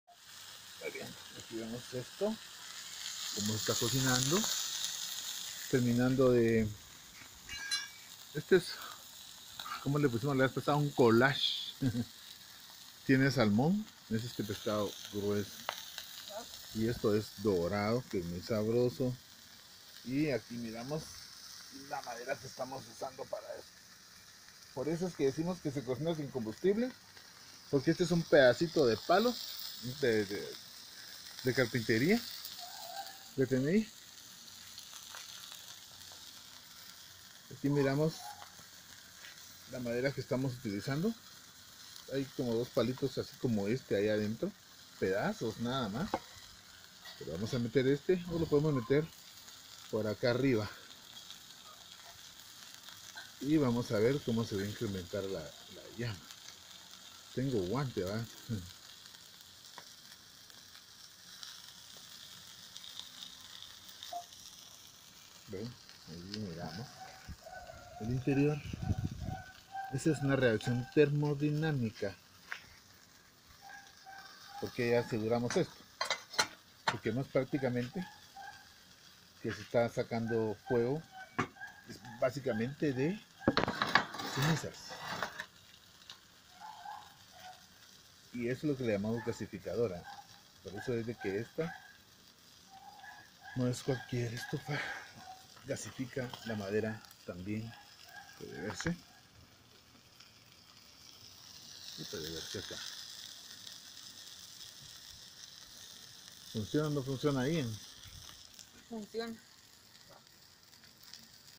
IanWil Survive Drive Collage de Salmón y Pez Dorado Guatemala 2,018
Comer saludable y con bajo costo es muy importante para la salud y el ambiente en este caso salmón y pez dorado